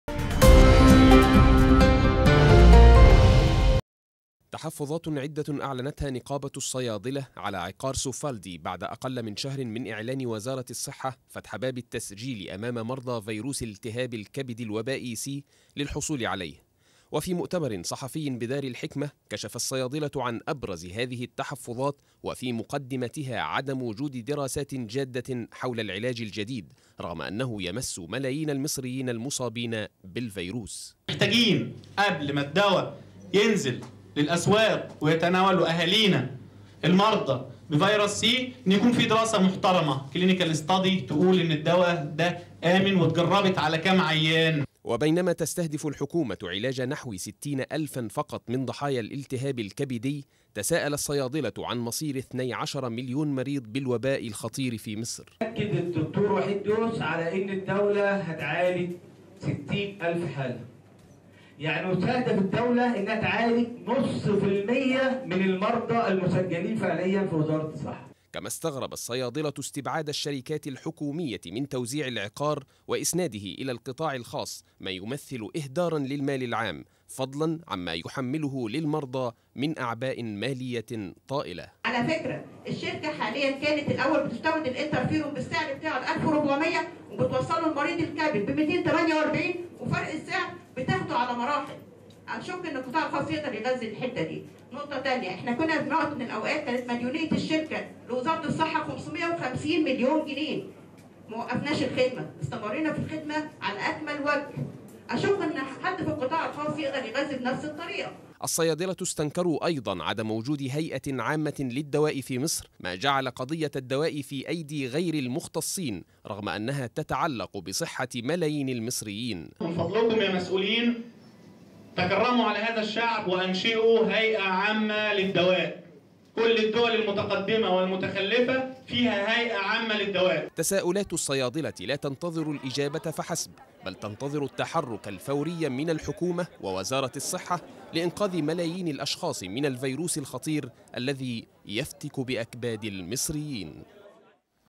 نقابة الصيادلة تطالب بإنشاء هيئة لمراقبة الدواء في مصر
متابعة إخبارية
٢-١٠-٢٠١٤